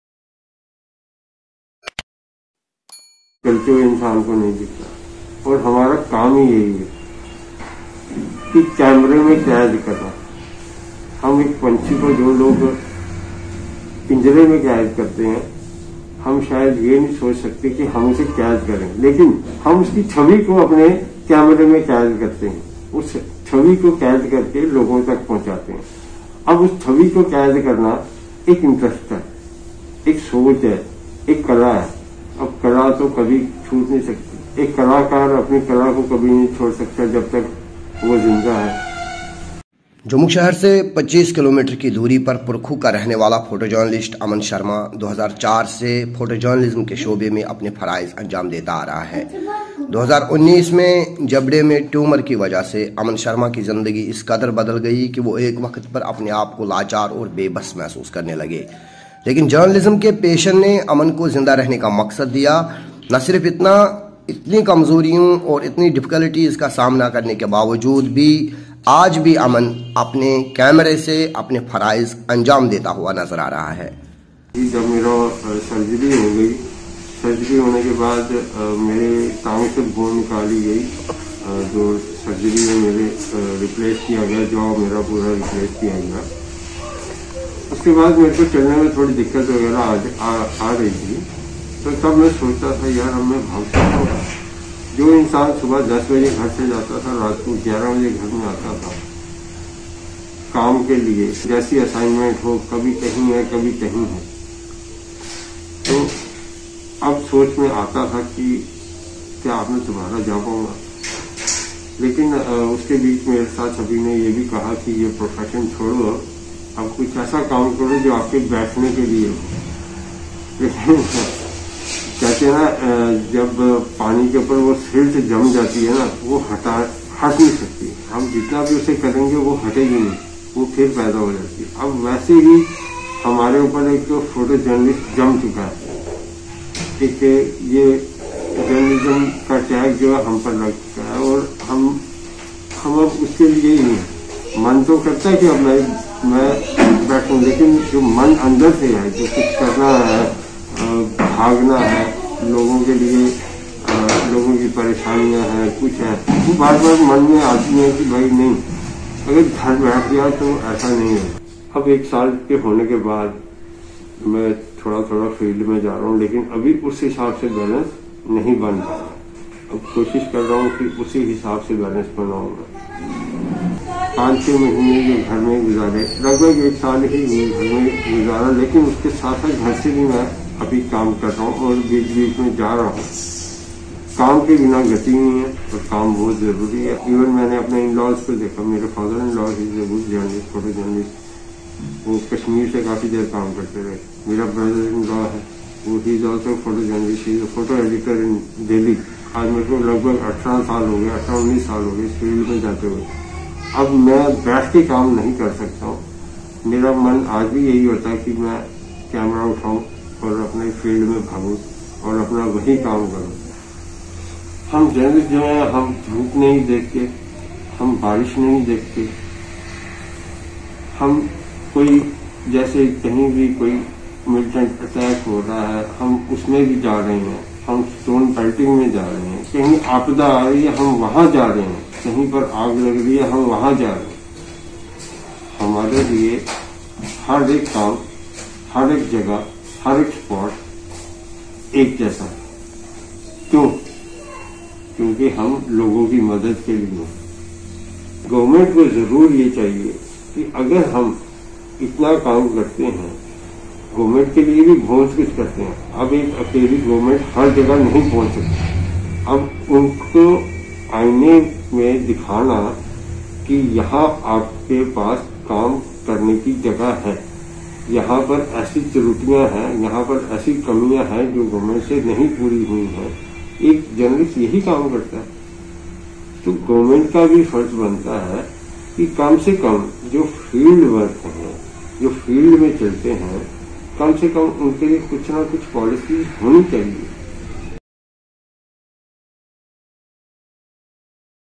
Keeping the Passion Alive in Difficult Times - Meet Aman, a Photojournalist
Belonging to an economically weaker family, Aman Sharma has been working as a photojournalist with several local newspapers across Jammu. Aman suffered malignant disease in his mouth in 2019, however, he continues to follow his passion and career in photojournalism.

Community Correspondent Basharat Amin reports from Ganderbal, Kashmir for Video Volunteers.  Learn more about Basharat and see other videos of his/hers